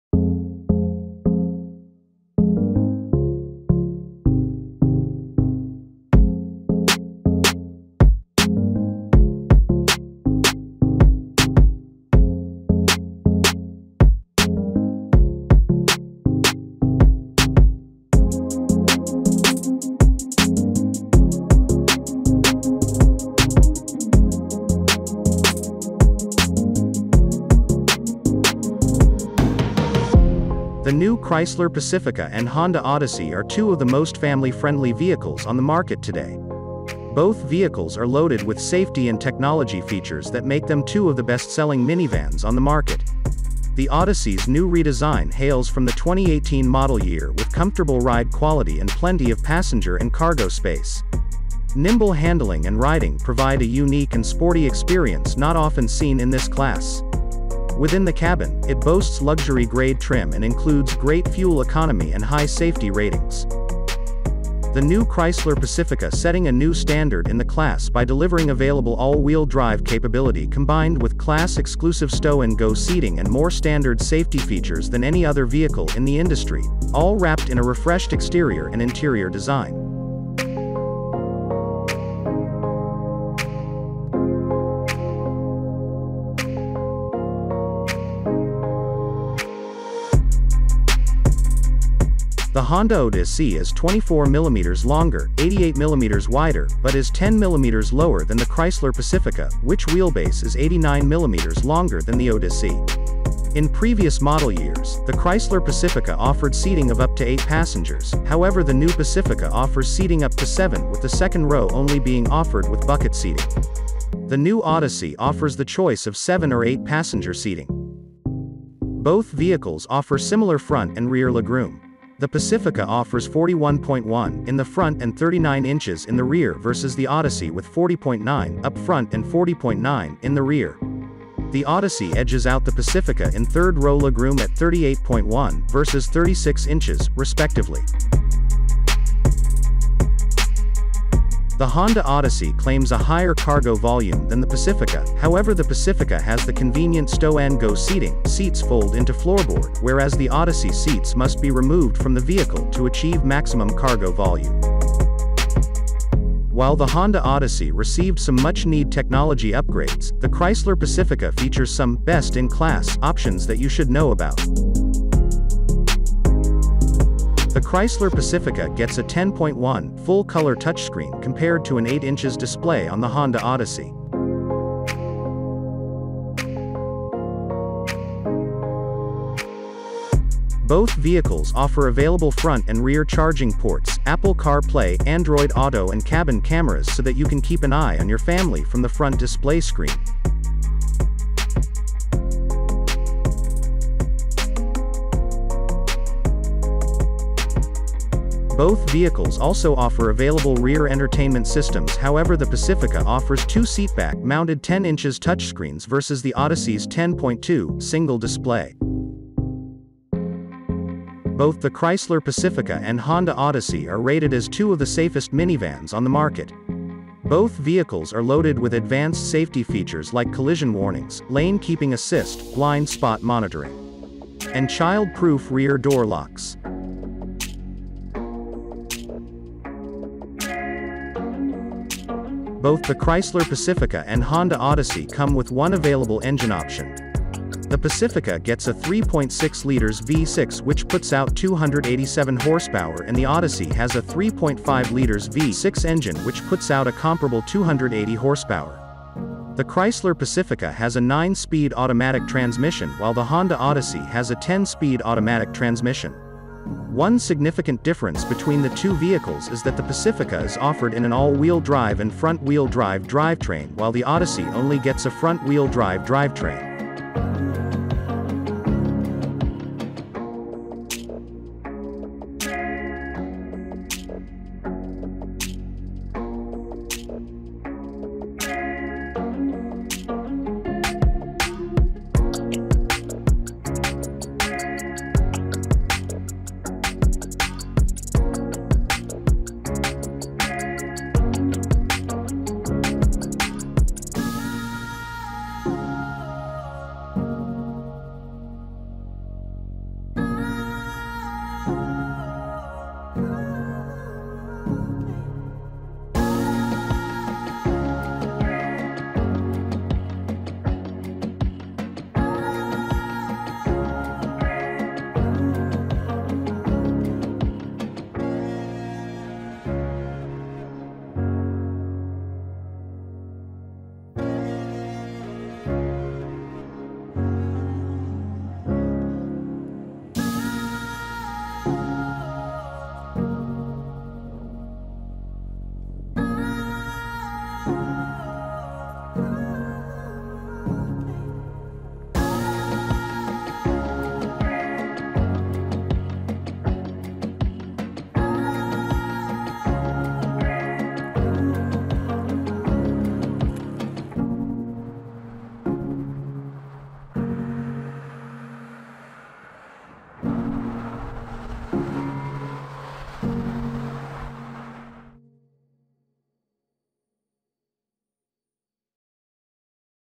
2021 Chrysler Pacifica vs 2021 Honda Odyssey | Family Minivans
2021 Chrysler Pacifica vs 2021 Honda Odyssey
Comparison Visual Driving, Exterior & Interior Design

The Chrysler Pacifica and Honda Odyssey are two of the most family friendly vehicles on the market today. Both vehicles are loaded with safety and technology features that make them two of the best-selling minivans on the market.

The Odyssey's new redesign hails from the 2018 model year with comfortable ride quality and plenty of passenger and cargo space. Nimble handling and riding provide a unique and sporty experience not often seen in this class. Within the cabin, it boasts luxury-grade trim and includes great fuel economy and high safety ratings.

The new Chrysler Pacifica setting a new standard in the class by delivering available all-wheel-drive (AWD) capability combined with class-exclusive Stow 'n Go seating and more standard safety features than any other vehicle in the industry, all wrapped in a refreshed exterior and interior design.

The Honda Odissey is 24 mm longer, 88 mm wider, but is 10 mm lower than the Chrysler Pacifica, which wheelbase is 89 mm longer than the Odissey.

In previous model years, the Chrysler Pacifica offered seating of up to 8 passengers; however the 2021 Pacifica offers seating up to 7 with the second row only being offered with bucket seating. The 2021 Odyssey offers the choice of 7 or 8 passenger seating. Both vehicles offer similar front and rear legroom. The Pacifica offers 41.1” in the front and 39” in the rear versus the Odyssey with 40.9” up front and 40.9” in the rear. The Odyssey edges out the Pacifica in third-row legroom at 38.1” versus 36”, respectively. The Honda Odyssey claims a higher cargo volume than the Pacifica, however the Pacifica has the convenient Stow-N-Go seating (seats fold into floorboard) whereas the Odyssey seats must be removed from the vehicle to achieve maximum cargo volume.

While the 2021 Honda Odyssey received some much need technology upgrades, the Chrysler Pacifica features some “best-in-class” options that you should know about. The Chrysler Pacifica gets a 10.1” full-color touchscreen compared to an 8” display on the 2021 Honda Odyssey. Both vehicles offer available front and rear charging ports, Apple CarPlay/Android Auto and cabin cameras so that you can keep an eye on your family from the front display screen. Both vehicles also offer available rear entertainment systems however the Pacifica offers 2 seatback-mounted 10” touchscreens versus the Odyssey’s 10.2” single display.

Both the Chrysler Pacifica and Honda Odyssey are rated as two of the safest minivans on the market. Both vehicles are loaded with advanced safety features like collision warnings, lane-keeping assist, blind spot monitoring. And child proof rear door locks.

Both the Chrysler Pacifica and Honda Odyssey come with one available engine option. The Pacifica gets a 3.6L V6 which puts out 287 horsepower and  the Odyssey has a 3.5L V6 engine which puts out a comparable 280 horsepower. The Chrysler Pacifica has a 9-speed automatic transmission while the Honda Odyssey has a 10-speed automatic transmission. One significant difference between the two vehicles is that the 2021 Pacifica is offered in an All-Wheel Drive (AWD) and Front-Wheel Drive (FWD) drivetrain while the 2021 Odyssey only gets a FWD drivetrain.